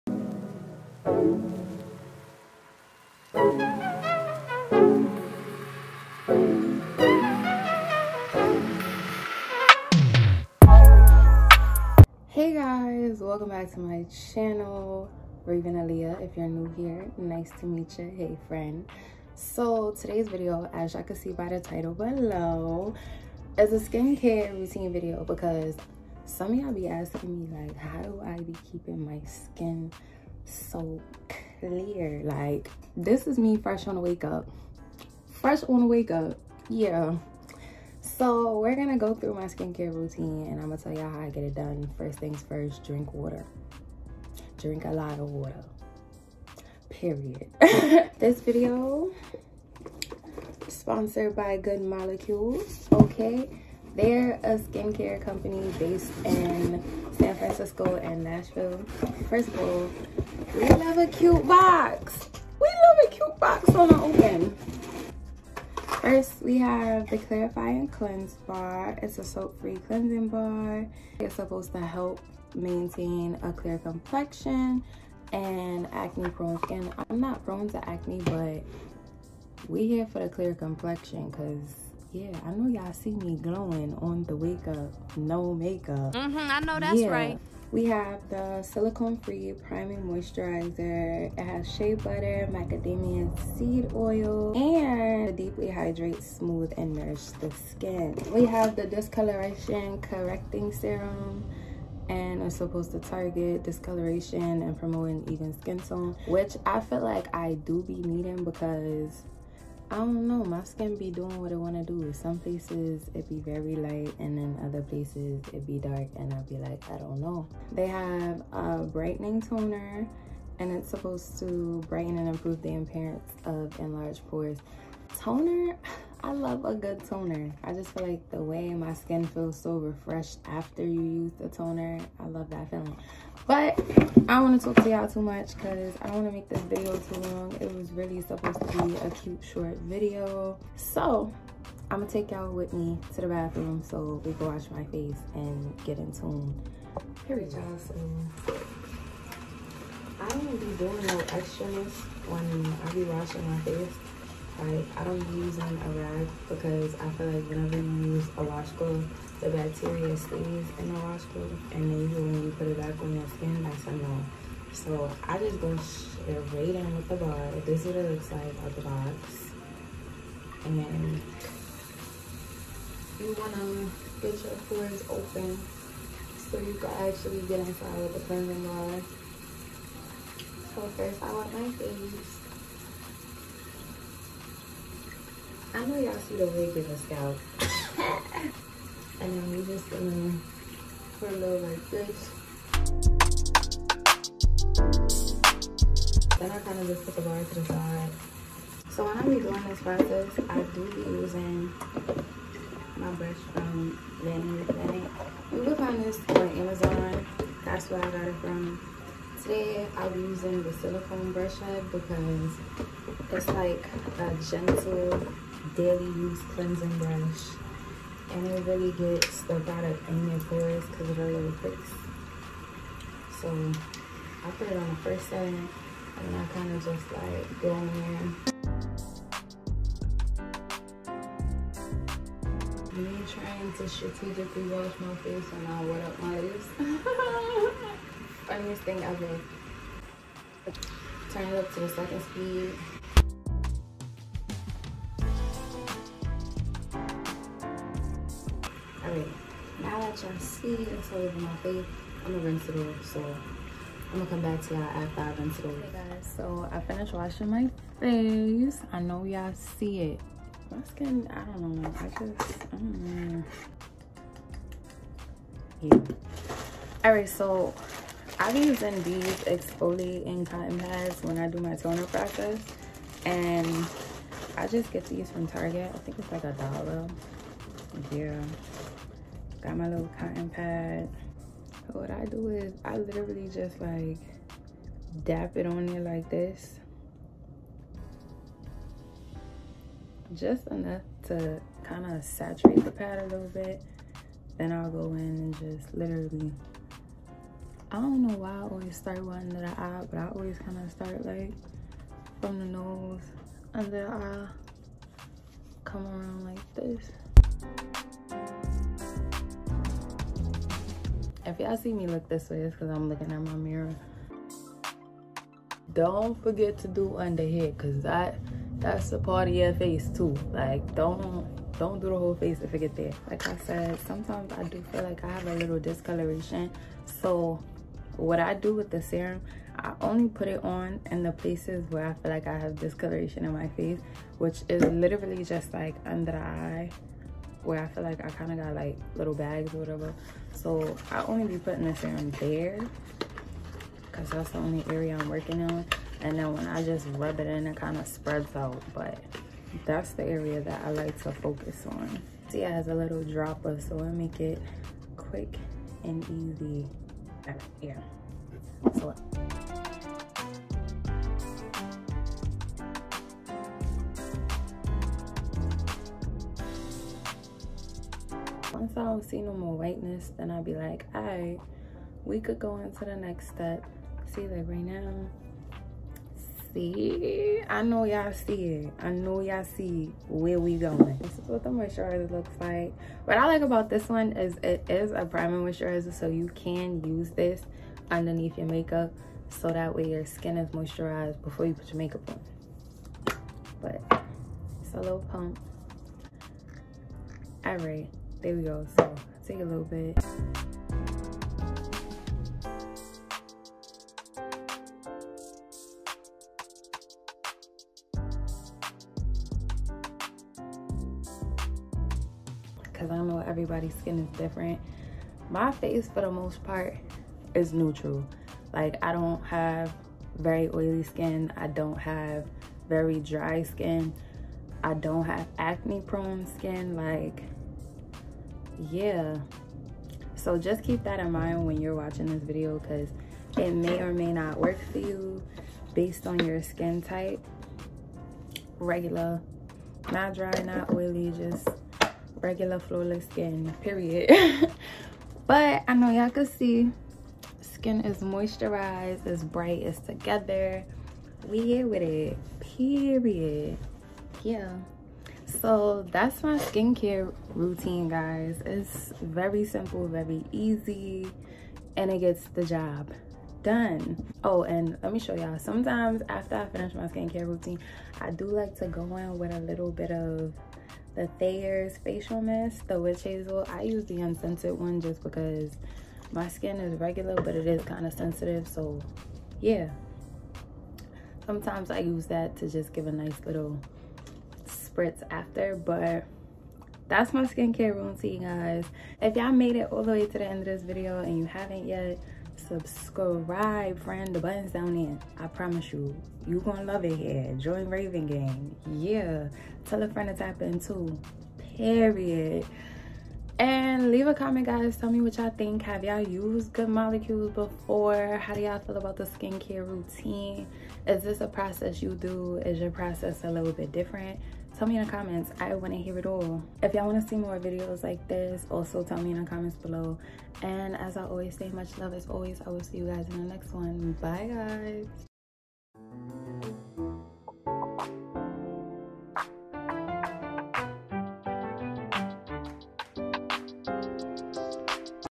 DAILY SKIN CARE ROUTINE FT. GOOD MOLECULES |Raven Aaliyah
Heyyyy Raven Gang!! Let me show y'all how I keep my skin clear and bright.

NEW VIDEOS WEEKLY TO BI-WEEKLY

A: 23

A: Brooklyn, NY

A: IPhone 12 Pro Max

A: Filmora X and IMovie